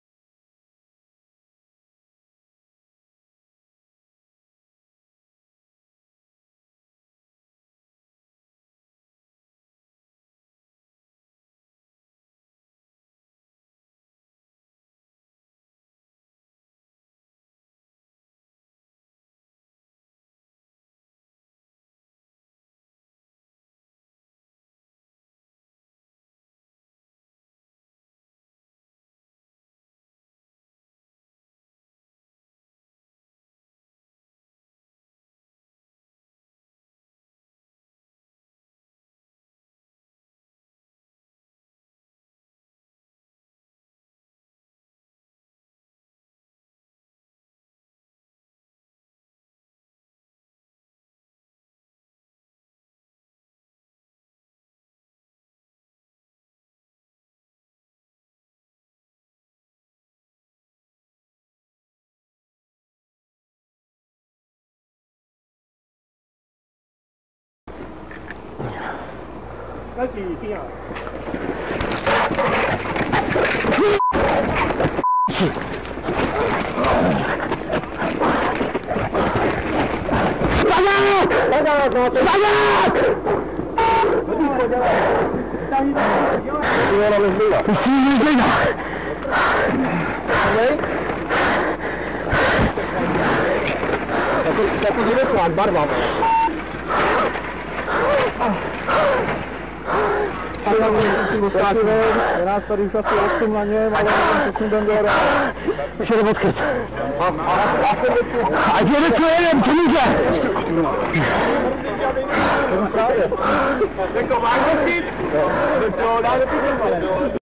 Runaway Wild Boar Terrifies Prague Shoppers
The boar was caught on CCTV running amok in the city centre
Police have captured a wild boar which swam across the Vltava river and terrified shoppers in the centre of Prague.